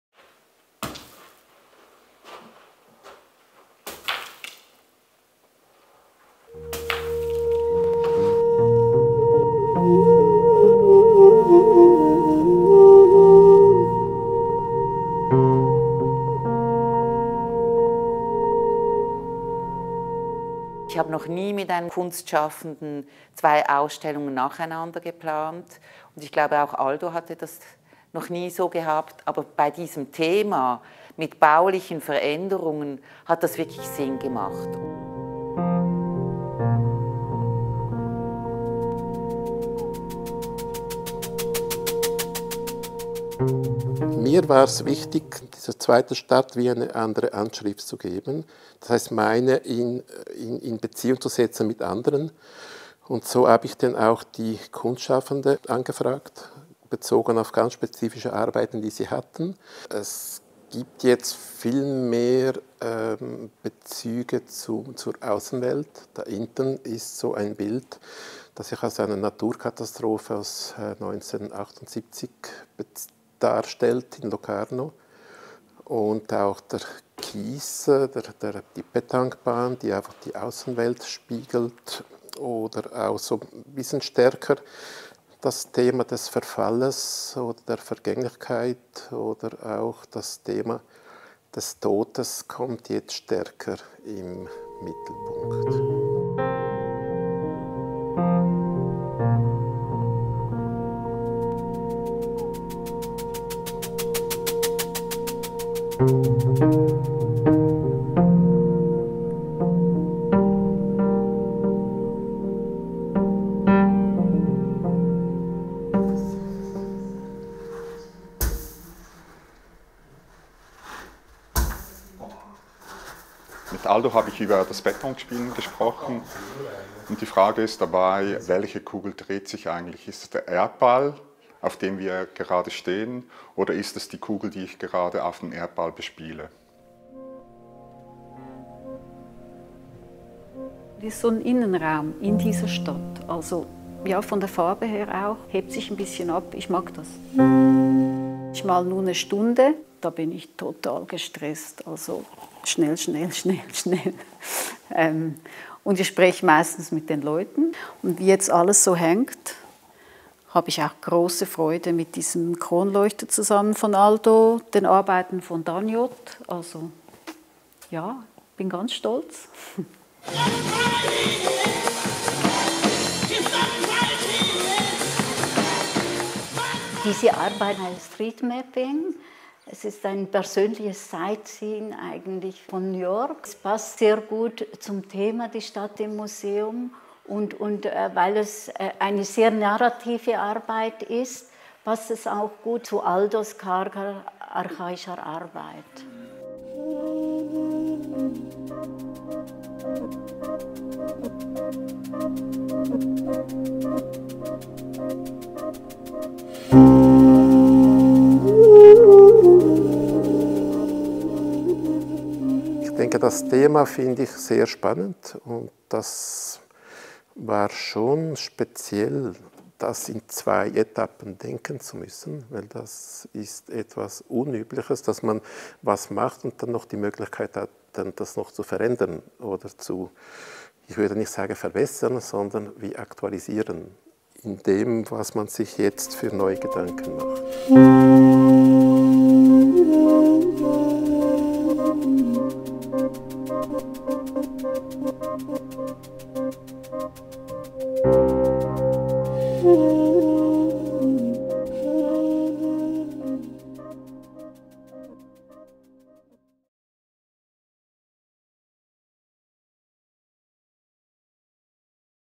Haus für Kunst Uri | Aldo Mozzini | Die Stadt im Museum - Part II
Nach erfolgreichem «Part I» hat der Künstler seine Stadt nun umgebaut. Wie jede Stadt ist sie im Wandel und nimmt aktiv auch die Einflüsse anderer Kunstschaffenden auf. Acht eingeladene Künstler*innen beleben die zweite Stadt und treten mit ihr in Austausch. Von Innen nach Aussen und von Aussen nach Innen. Ein Auftritt, in dem Spiel und Tod zusammenkommen. Die zweite Stadt ist freier, leerer – voller Räume für Gefühle und Geschichten.